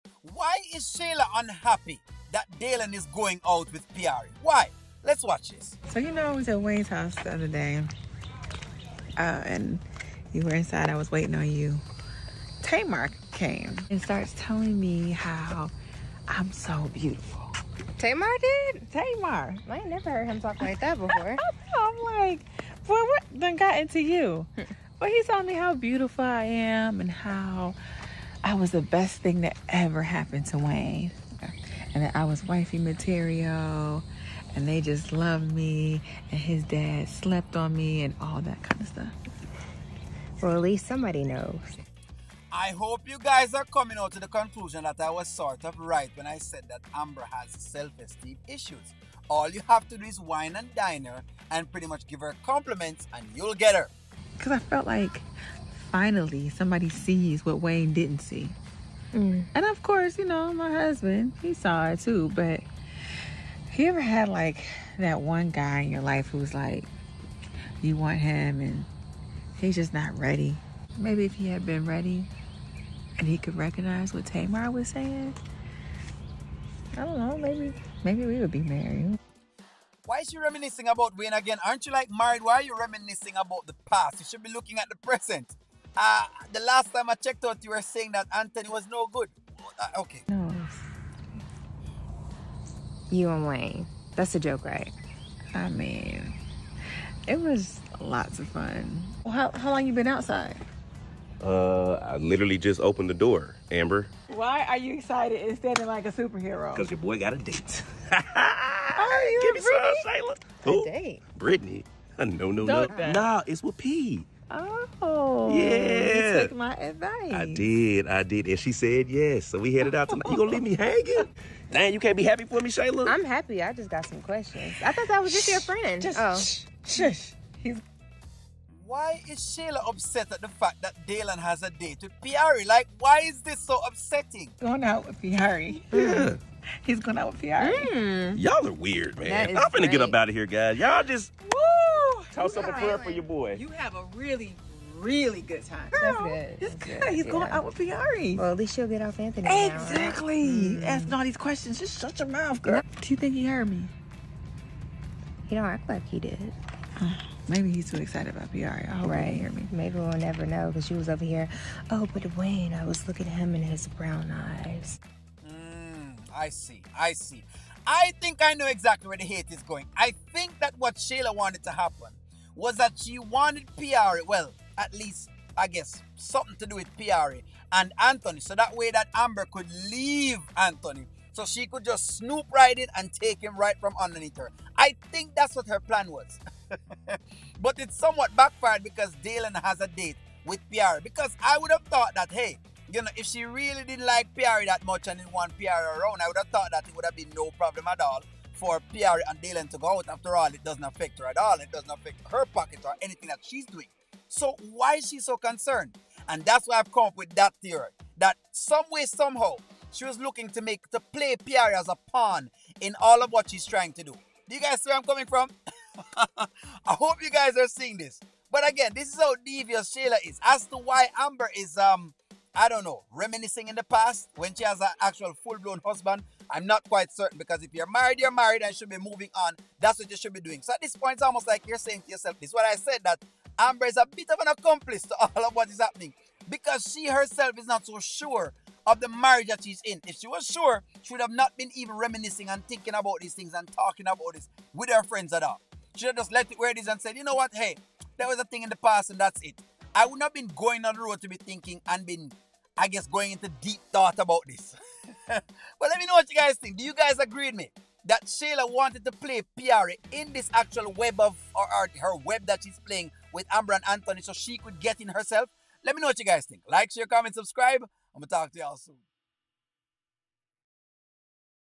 Shayla, you want Dalen to hear me talking about Wayne? - Kounty Wayne
Was Shayla trying to let Dalen hear Amber talking?